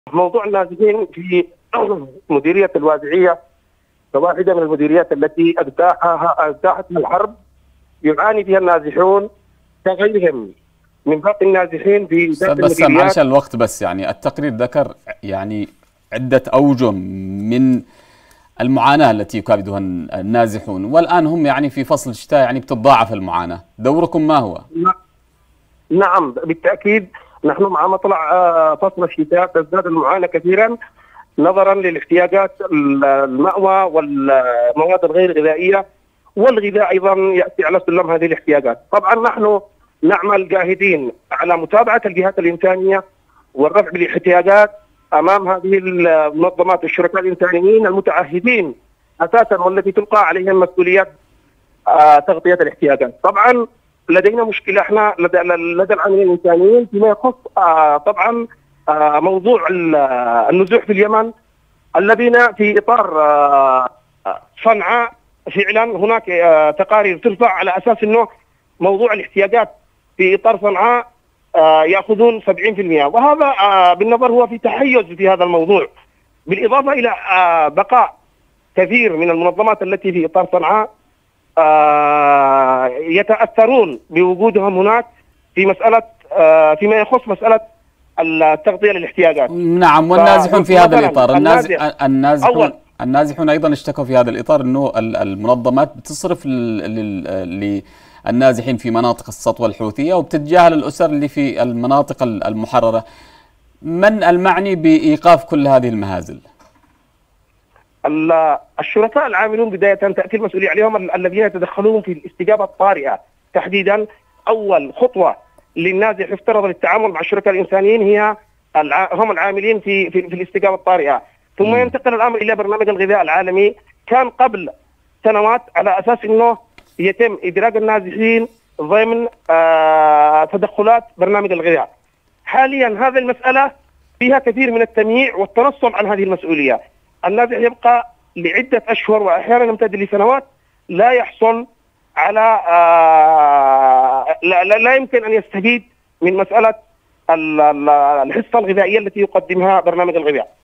بسام الحداد : تتزايد احتياجات النازحين في الشتاء للمأوى والمساعدات الإغاثية
مدير الوحدة التنفيذية لشؤون النازحين في تعز

تليجرام